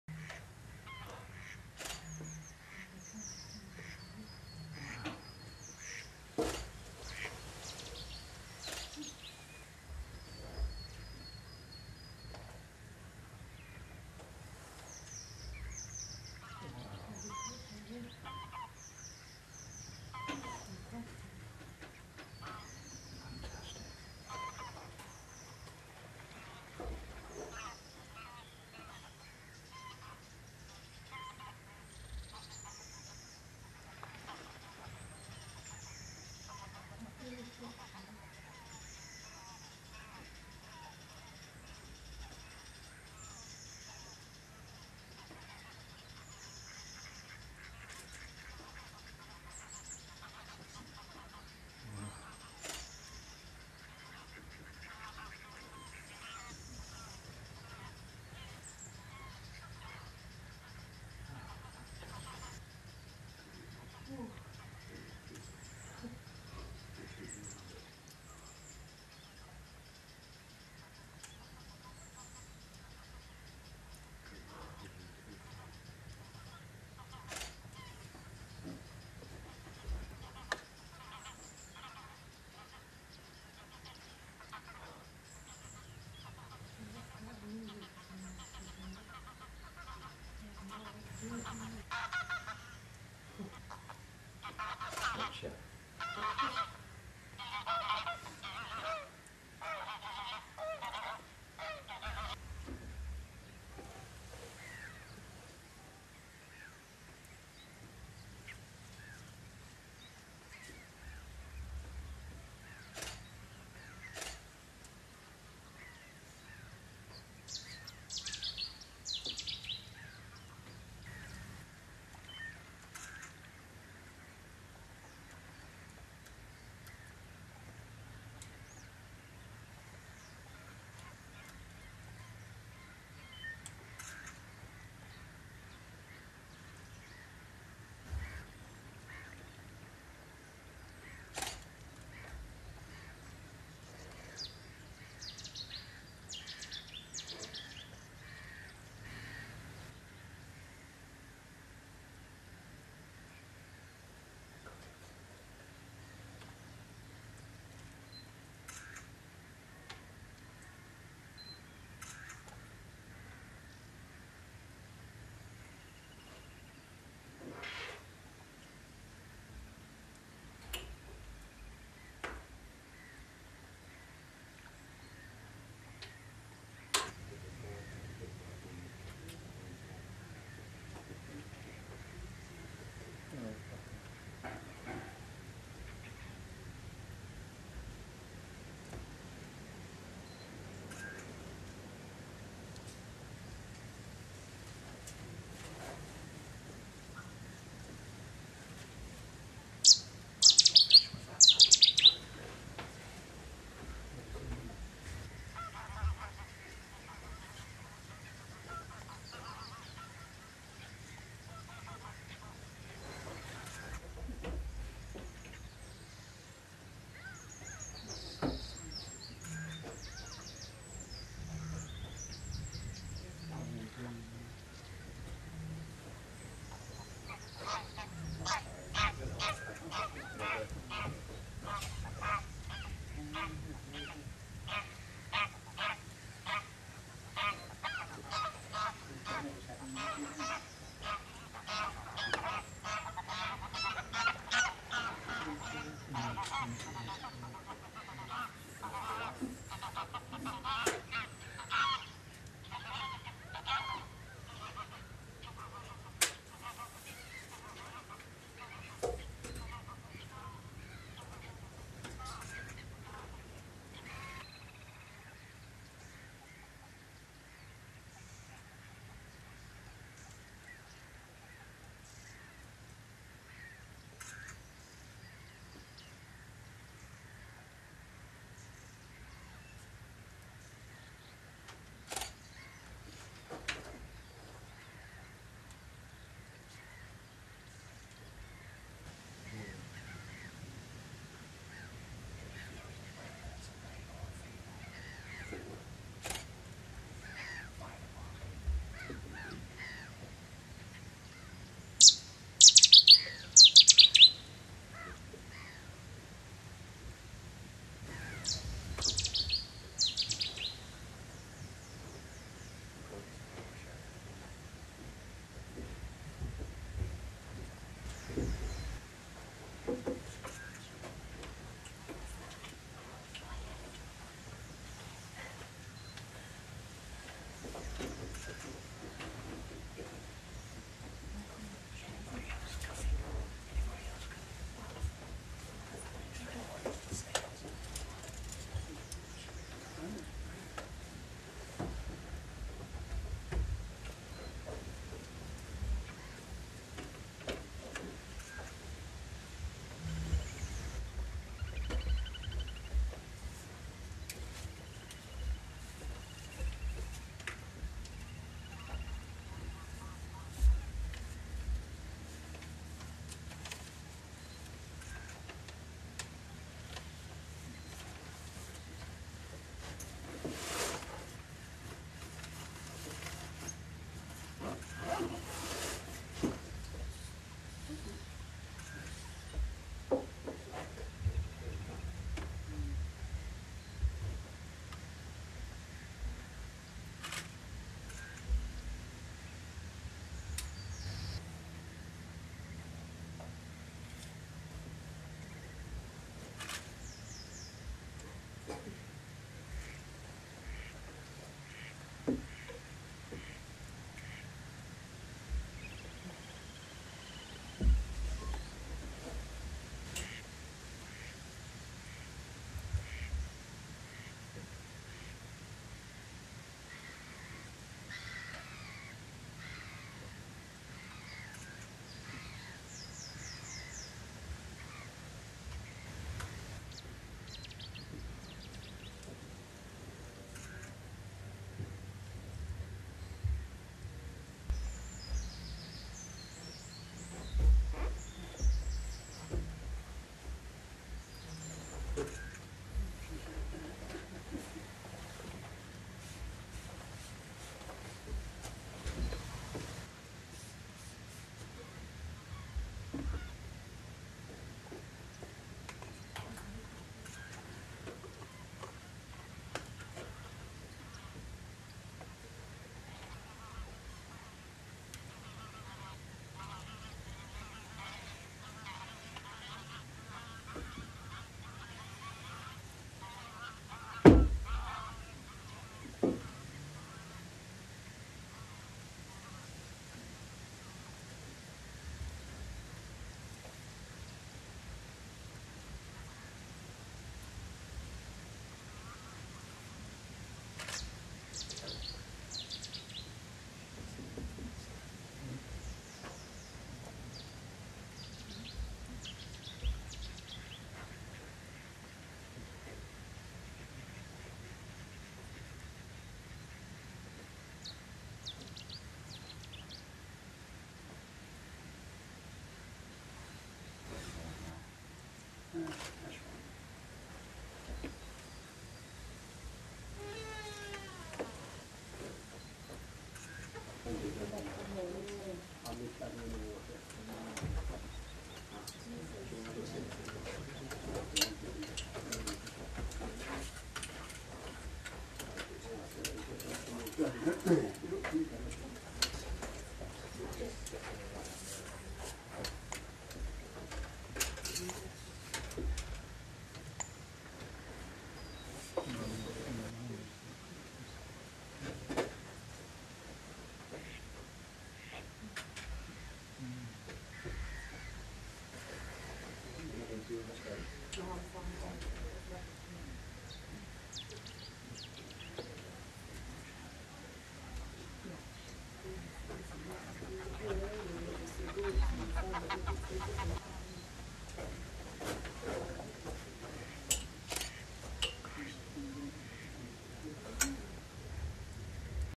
Bittern
Bittern fishing at Minsmere. 18 April 2010.
ibirdz.co.uk